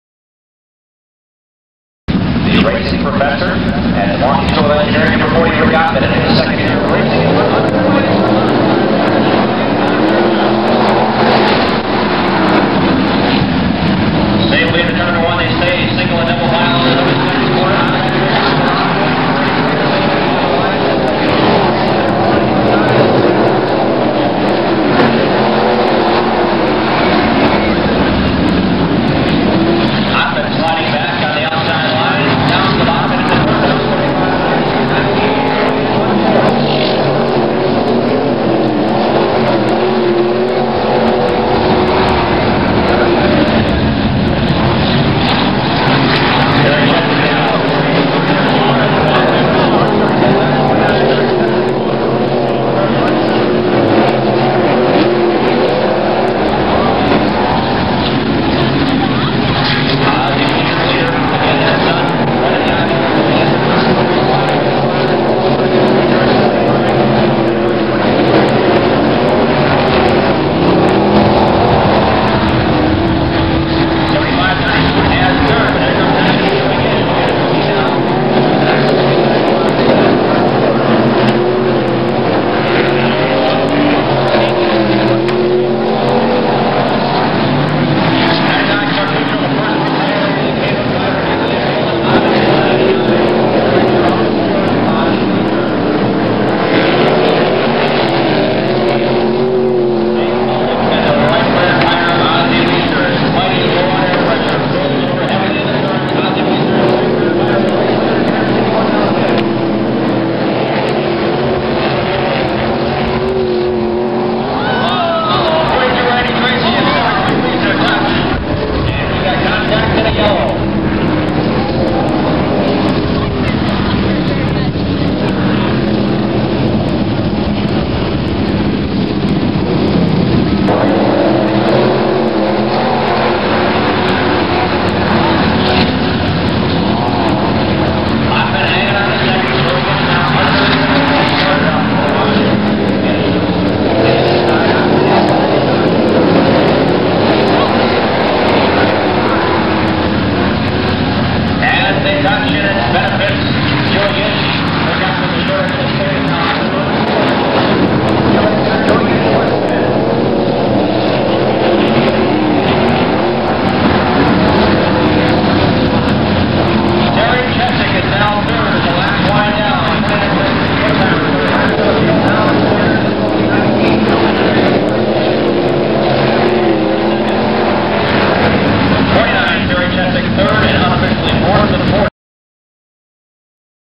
EWSC Plymouth 6-22-1991 Heat Race
#73 Ozzie Wesener
#40 Rick Ottman
#42 Randy Tracy
#2 Richard Berth
#99 Chuck Tuschl
#29 Terry Chesak
#75 Don Smoot Jr. & Brian Gilles
#19 Joey Gish
#15 Greg Otte
#63 Jay Schneider